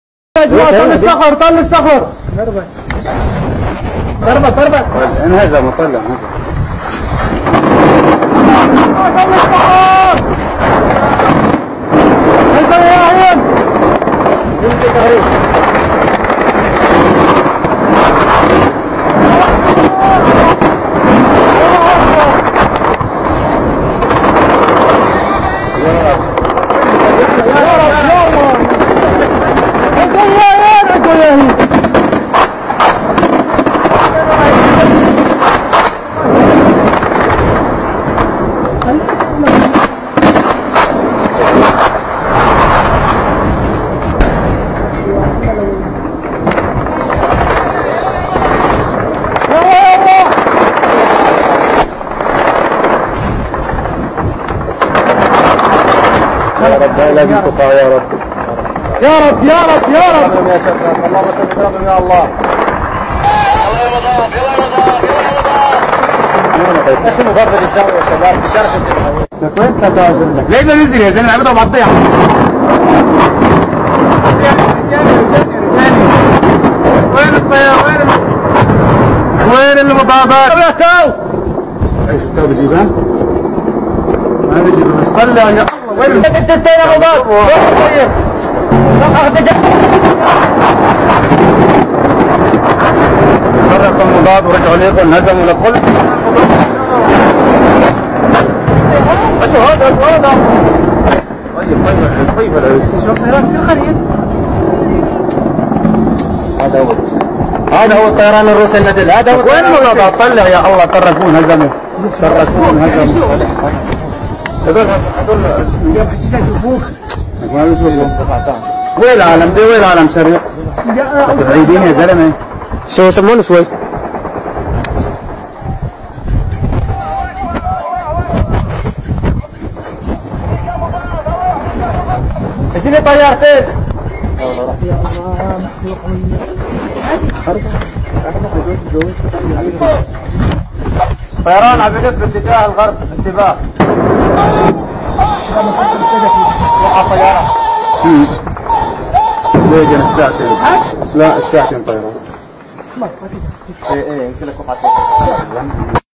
RuAF Hinds taking on tangos in Syria!!!
An epic video showing Russian Mi-24s providing ''Close'' Air Support in Syria.

The Flying Tanks can be seen raising dust, letting loose hell on terrorists. Some skilled gunship pilots with balls of Titanium here.